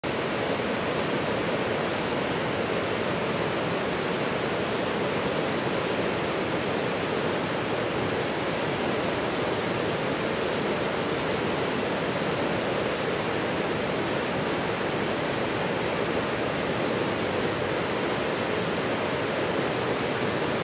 So many Coccinellidae (ladybugs!) Wawona is Awesome!
We found them near the South Fork of the Merced River in Southern Yosemite.
There are so many wonderful spots in Wawona!